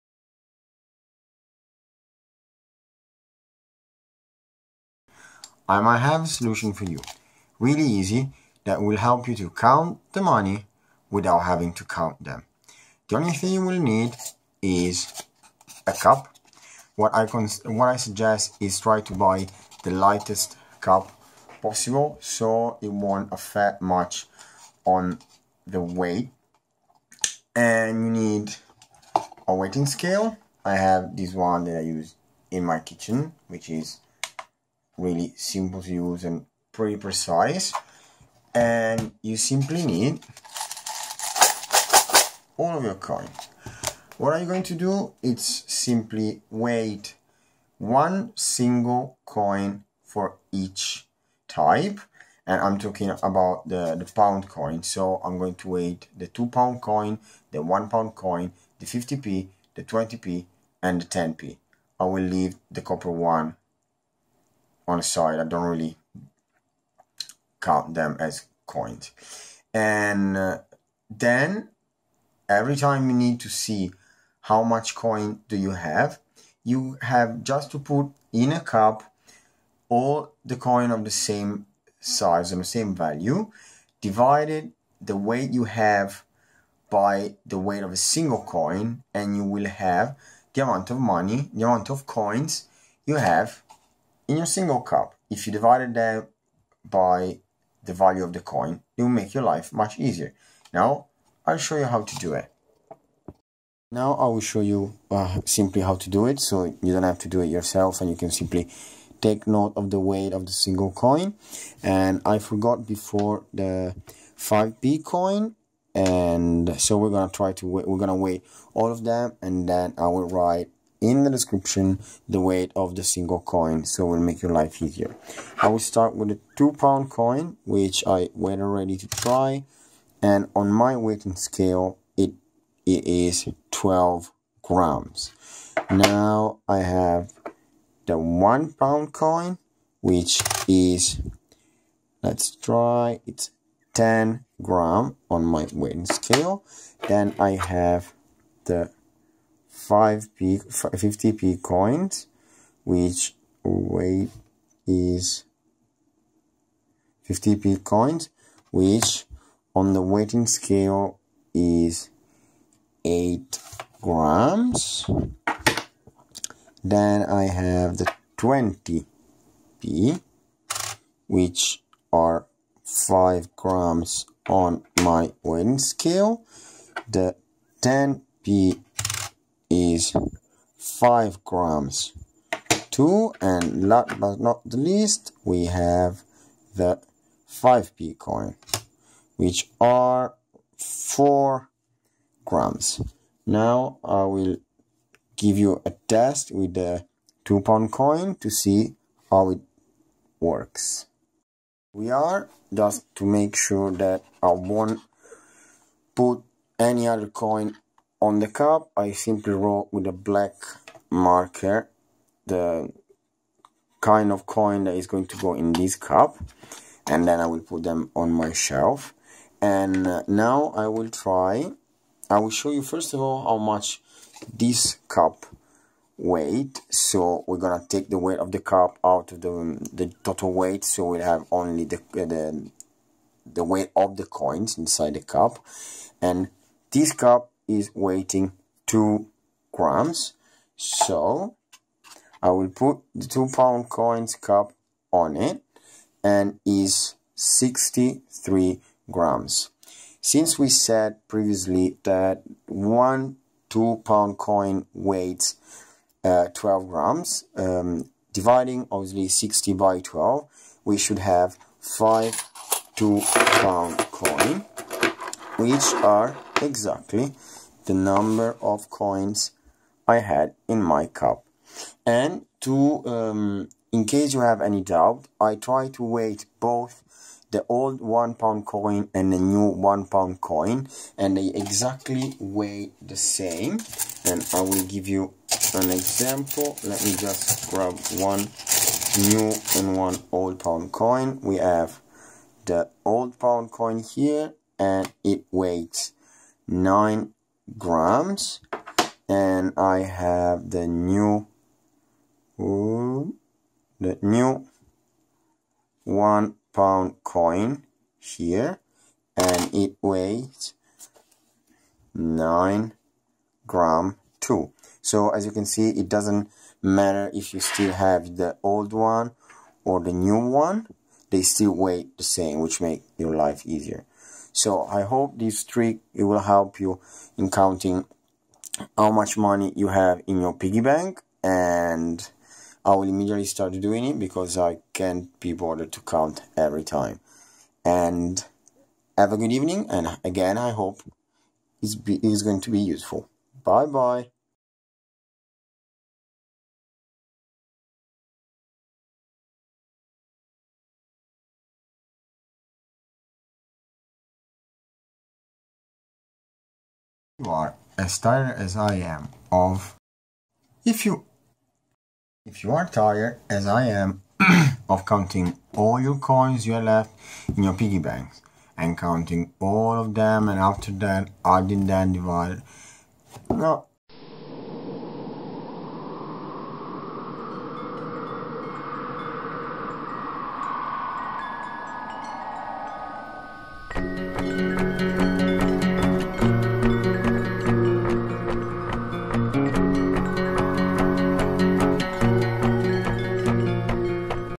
How to count coins without counting them.
Weight of the single coins:
£2: 12gr
£1: 10gr
50p: 8gr
20p: 5gr
10p: 5gr
5p: 4gr